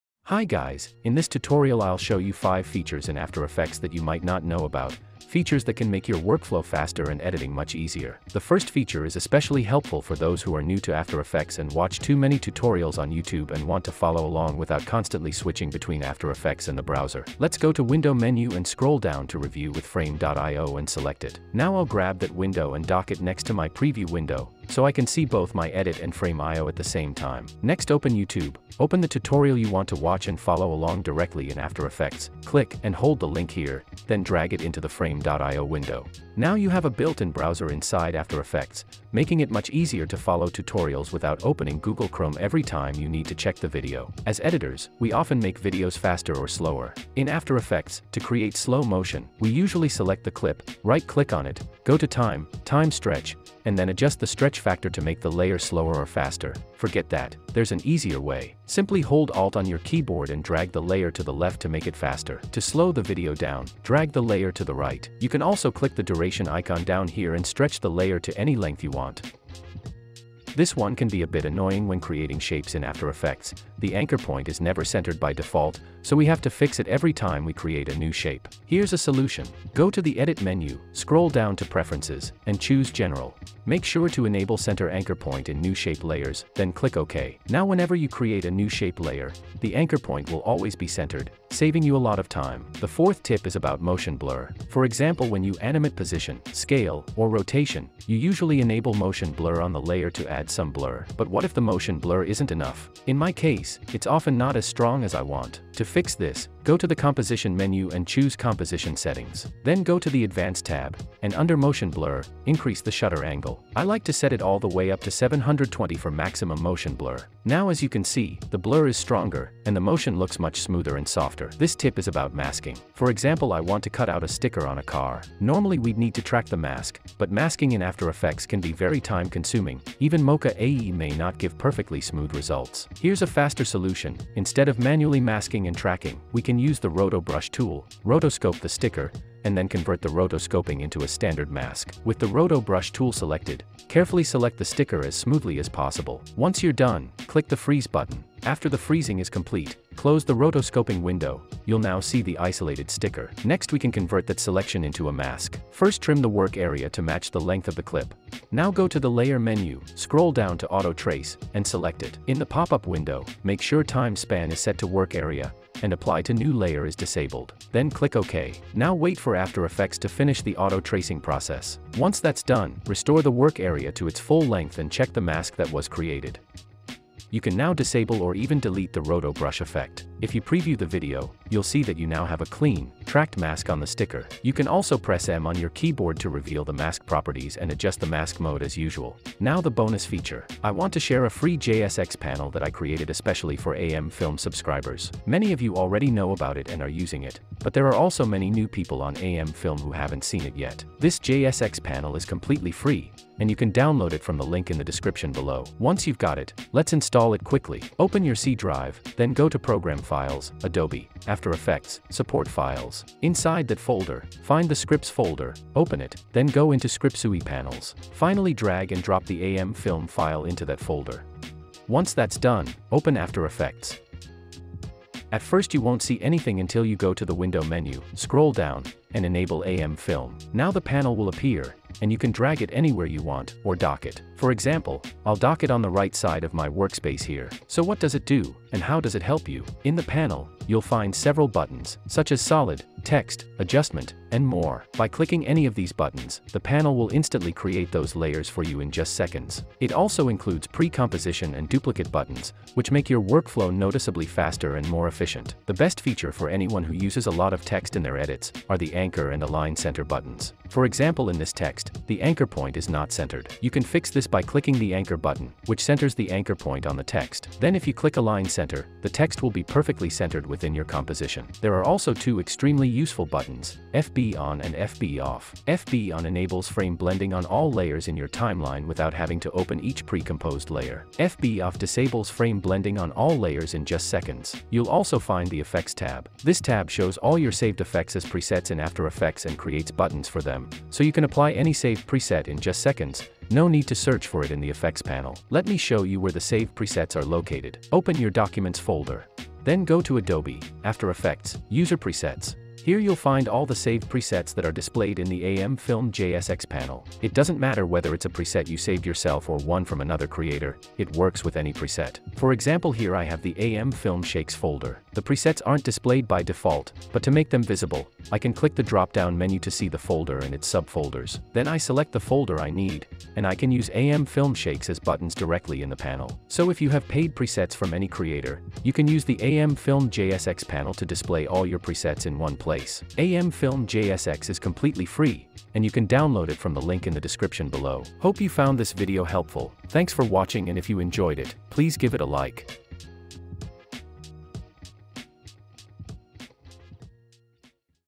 5 Things You Didn't Know About After Effects 🤯✨
In this tutorial I will show you 5 things and features you didn't know in Adobe After Effects

Chapters
0:08 - Frame.io (YouTube in AE)
0:44 - Stretching Clips
1:10 - Center Anchor Point
1:32 - Motion Blur
2:01 - Easy Masking
3:09 - Bonus - AM Film JSX

CPU: i7-14700K
GPU: RTX 4060 Ti 16GB
RAM: Kingston FURY Beast 32GB